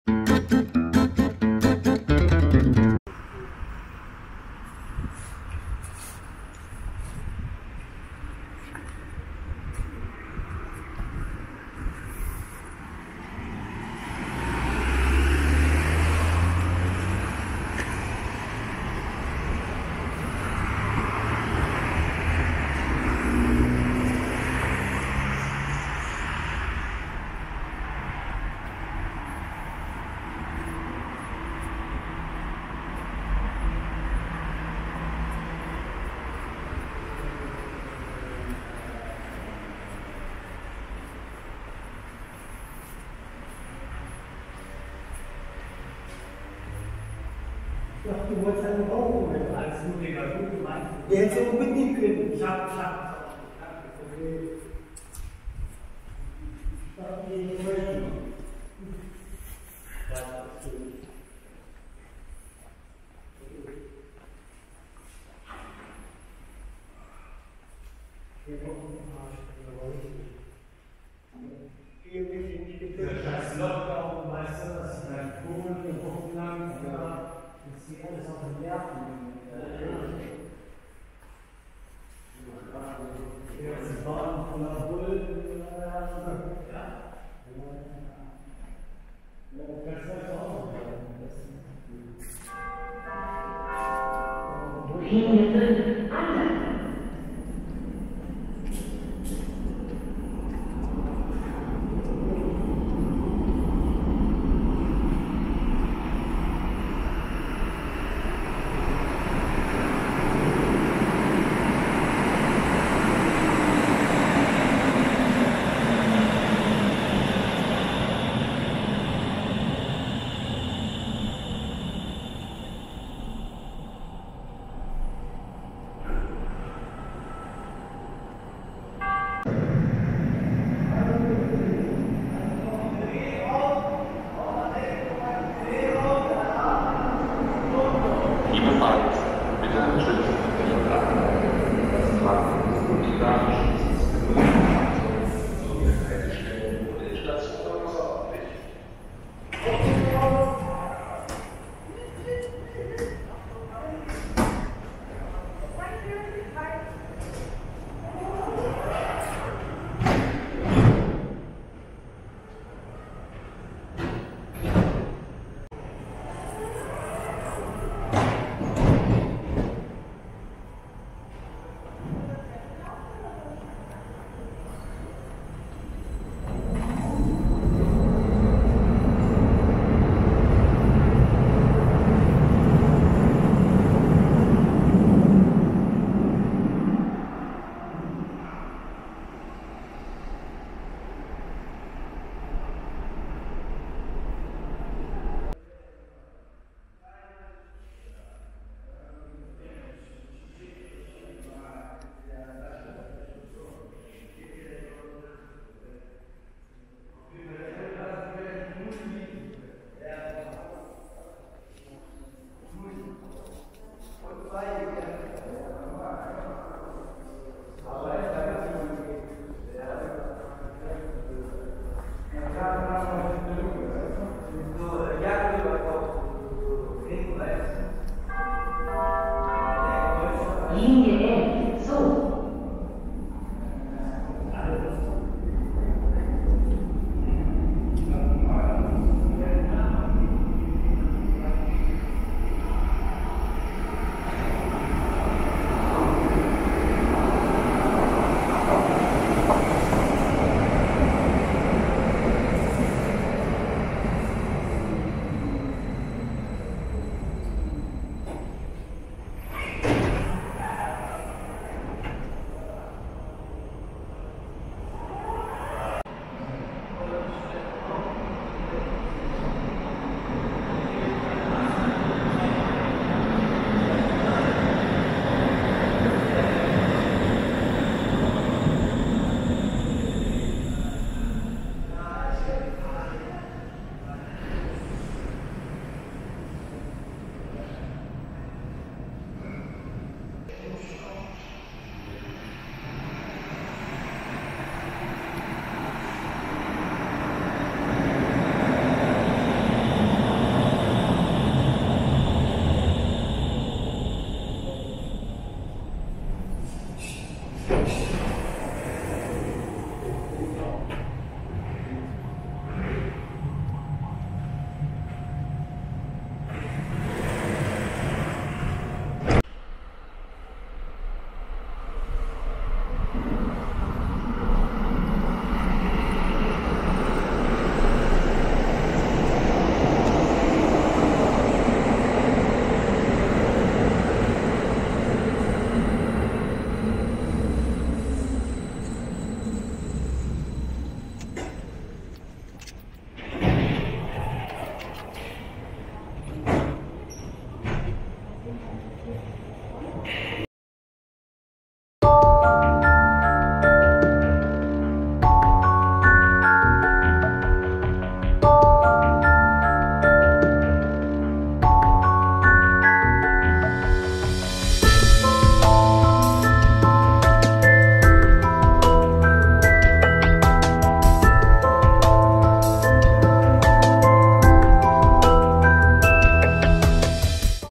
Stadtbahn Station Braunschweiger Platz - Hannover 🇩🇪 - Walkthrough 🚶
Braunschweiger Platz is a station on the lines 4, 5, 6 and 11.
The station opened on September 24, 1989.